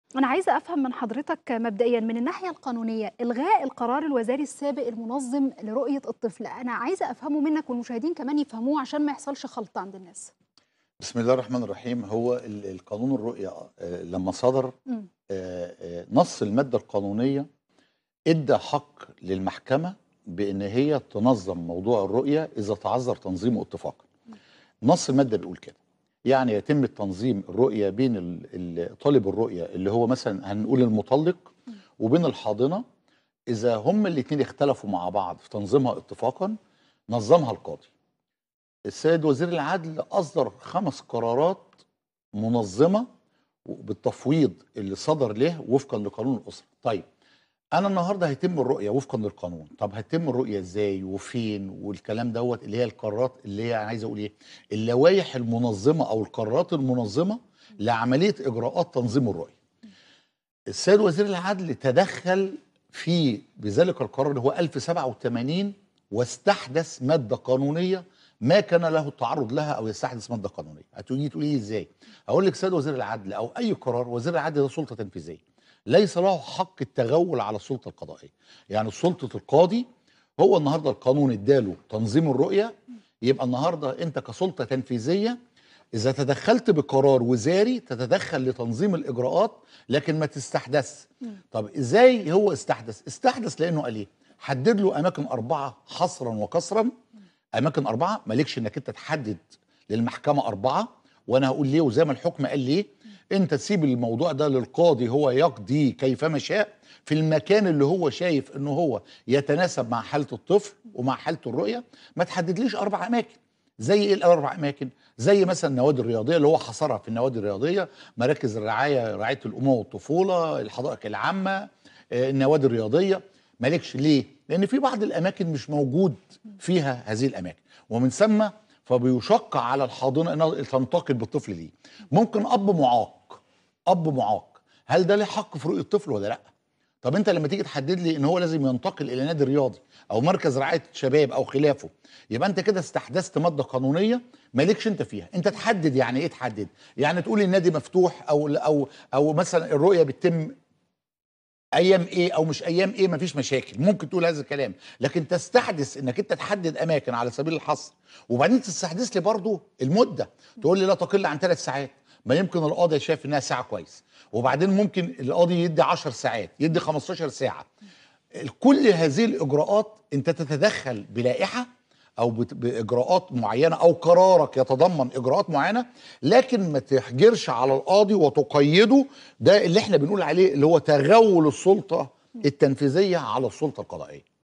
الجمعة_في_مصر | عصام عجاج المحامي بالنقض يشرح قانونياً الضوابط المنظمة لرؤية الطفل
عصام عجاج المحامي بالنقض يشرح قانونياً الضوابط المنظمة لرؤية الطفل